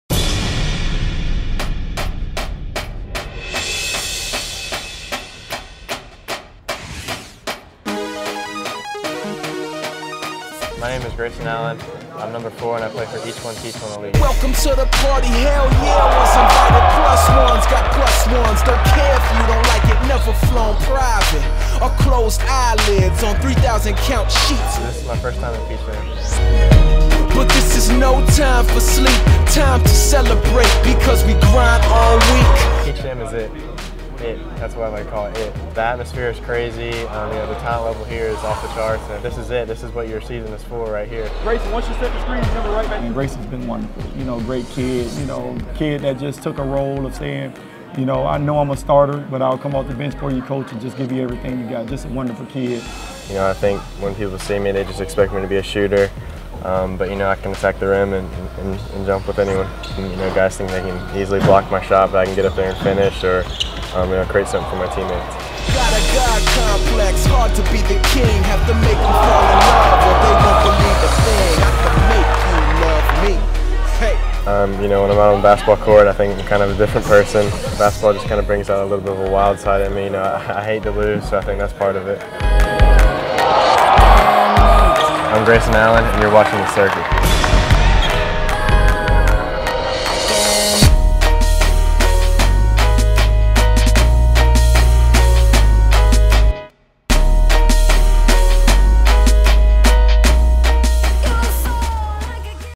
GRAYSON ALLEN LAUNCHING OFF AT PEACH JAM
After Day 2 of the NIKE EYBL Finals at the Peach Jam, E1T1 has locked a spot in the Quarterfinals with four wins... through the help of Grayson Allen. Grayson joined E1T1 before Dallas, and the team has been on a streak since.

Grayson Allen is ranked 27th for the Class of 2014, and has already committed to Duke University.

Follow EYBLTheCircuit for constant NIKE EYBL Coverage!